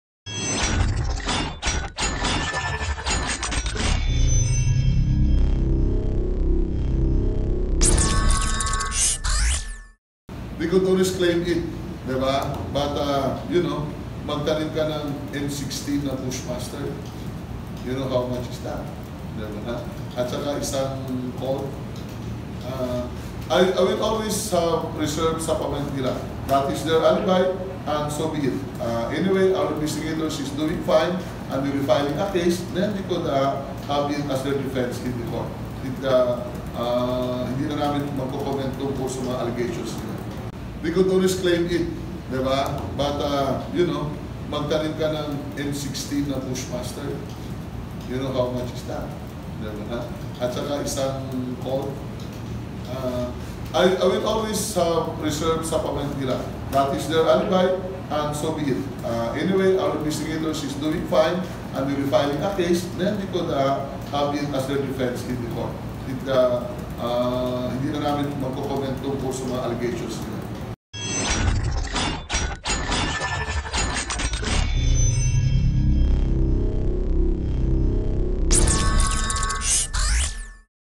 Tanim-ebidensya itinanggi ng NCRPO
Itinanggi ng NCRPO ang akusasyon ng mga militante na itinanim ng mga pulis ang dalawang baril na nakumpiska sa 3 suspek na kanilang kaalyado na inaresto ng PNP sa Tondo, Manila.